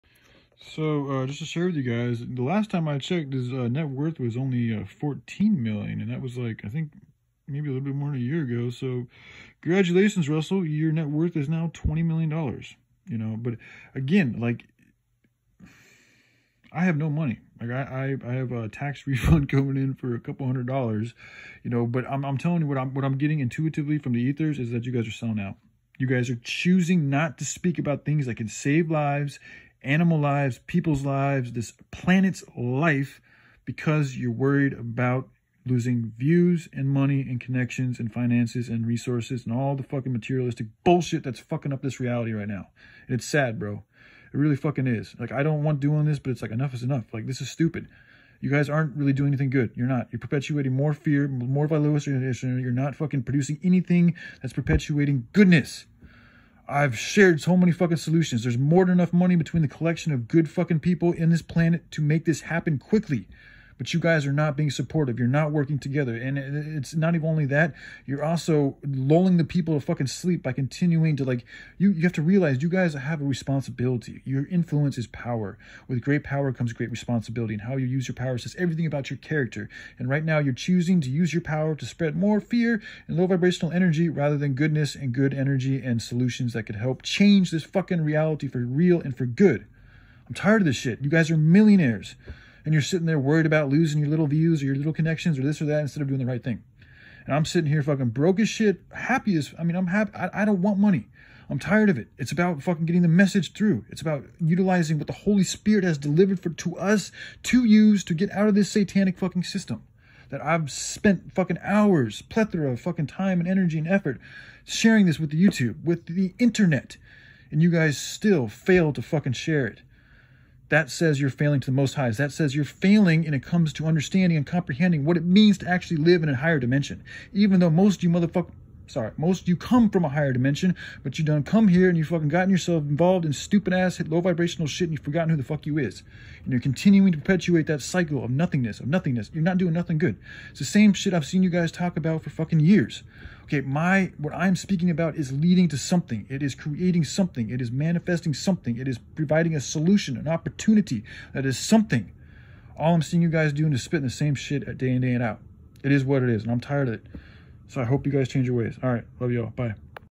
Russell has risen from a Net worth of 14 million 2 20 million…I’m sensing a sellout-I hope I’m wrong
13Love2All
13 Gratitude